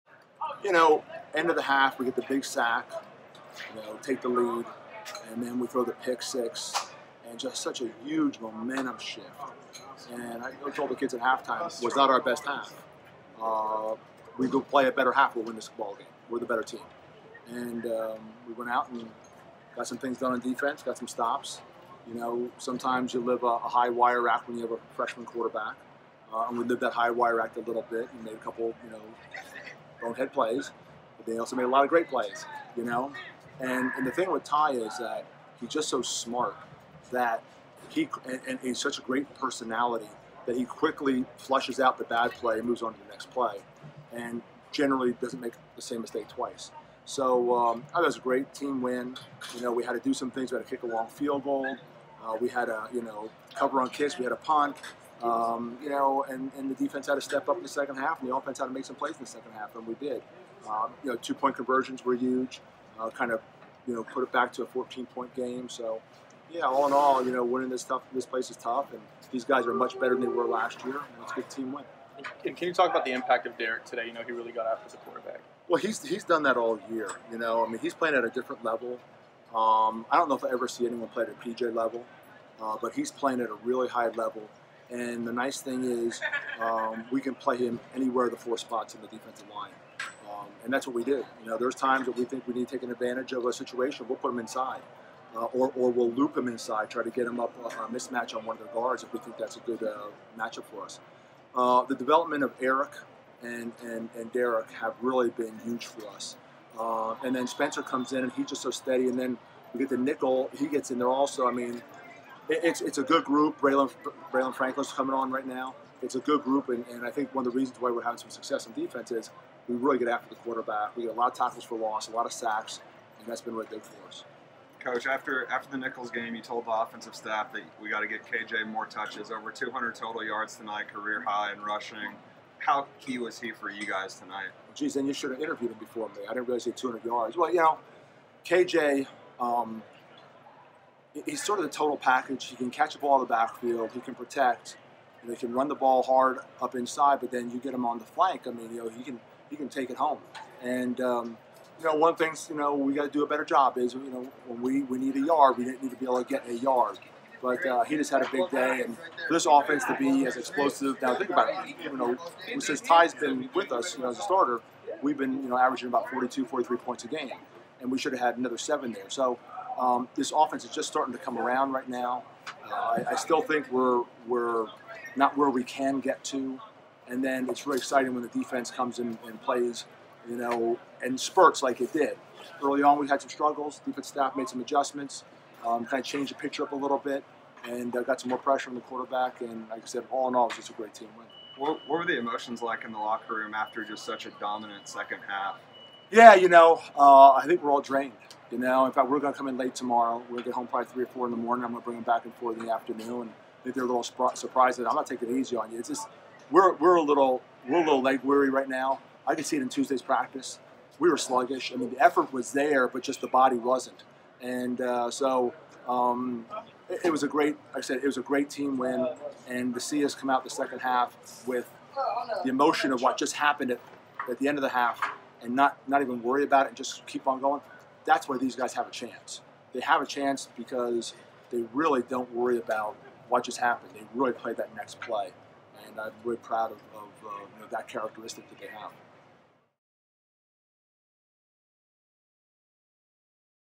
K.C. Keeler talks comeback win over NSU
Sam Houston State head coach K.C. Keeler discusses his team's comeback win over Northwestern State, Kyran Jackson's big night and more.